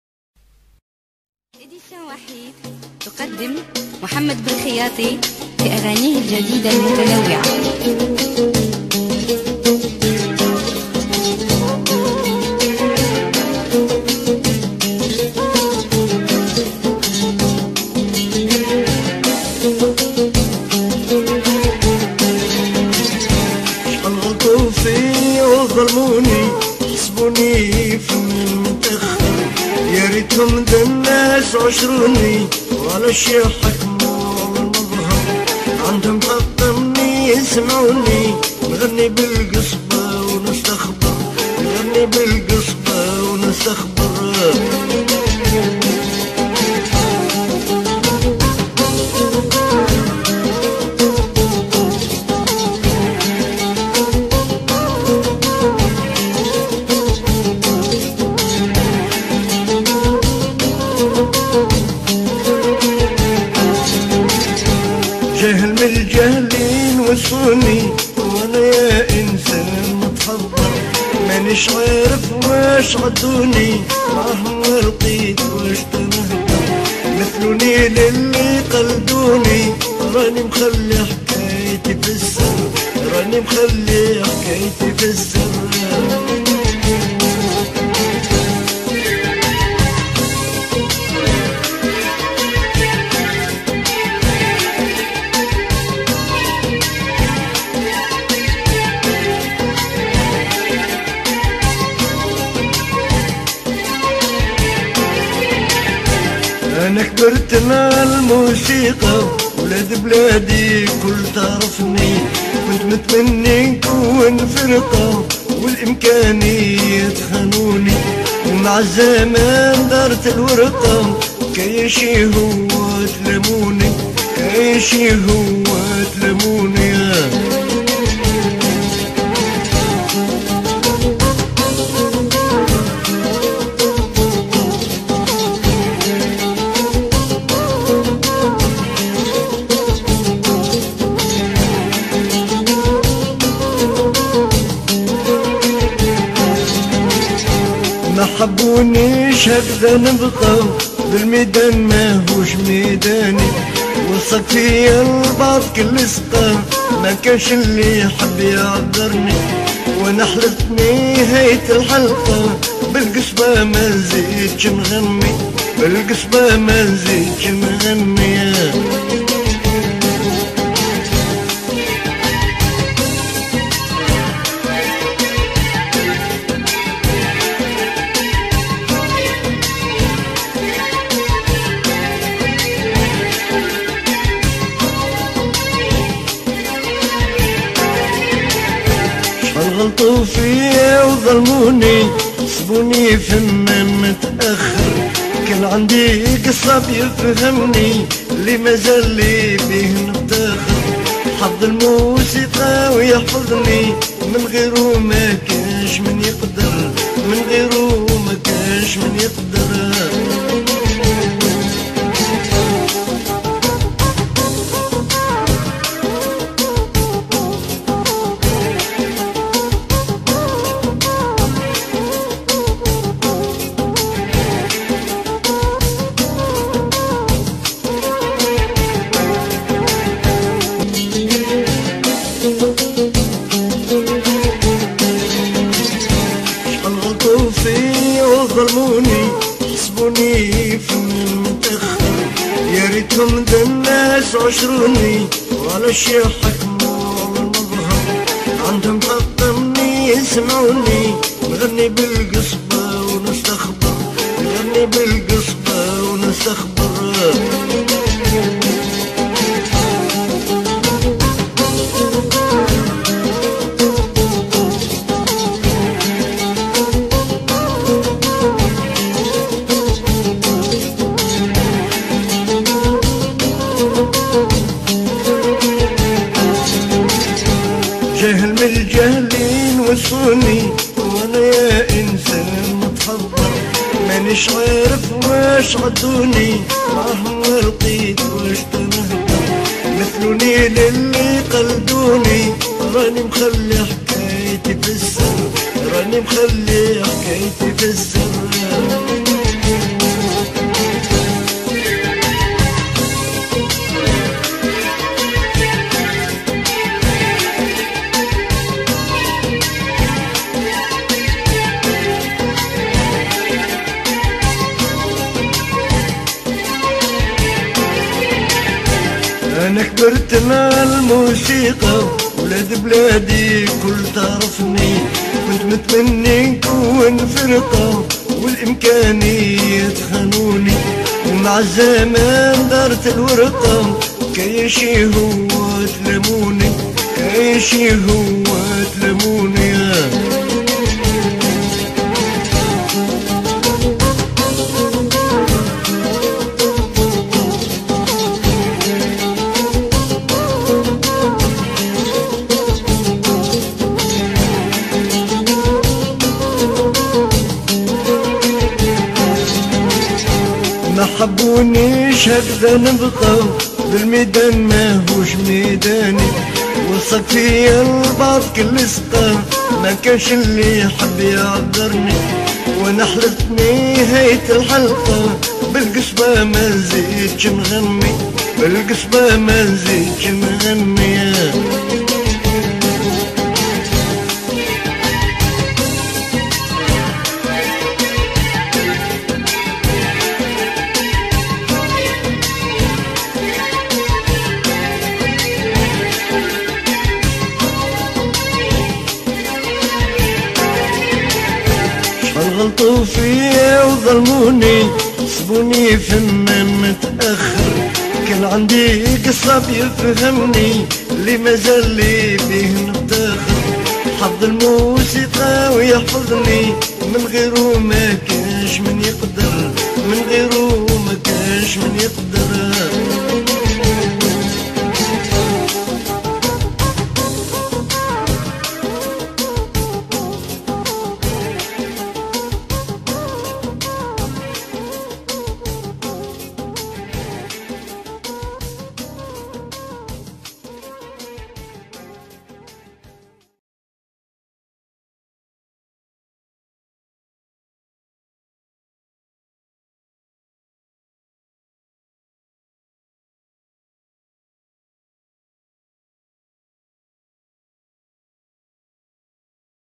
أغنية حزينة للشيخ محمد بلخياطي يبكي فيها على الشيخ النمس القصاب و رفيق الدرب الشيخ محمد بلخياطي
يمكنكم التواصل معنا عبر :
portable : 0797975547